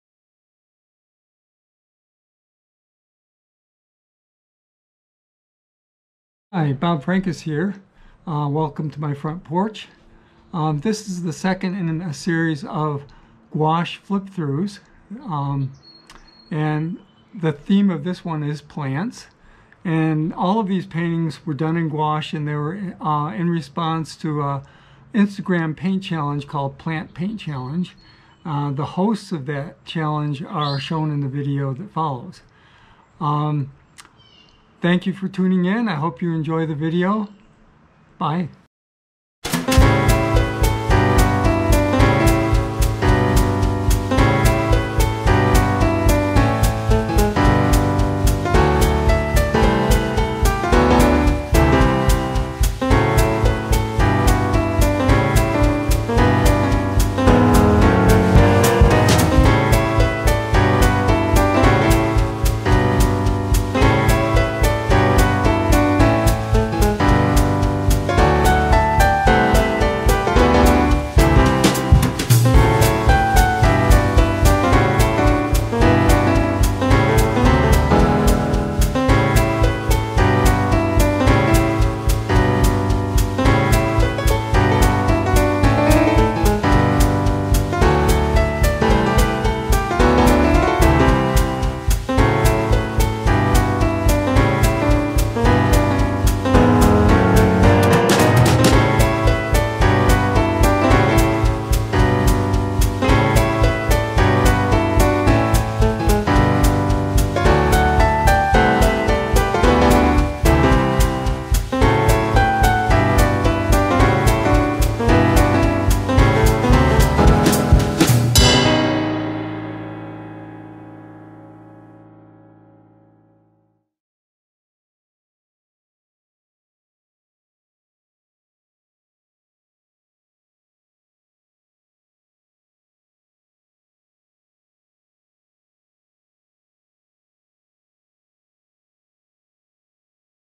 Painting Plants in Gouache
This is a flip-thru of gouache paintings that I did for an Instagram paint challenge called plantpaintchallenge.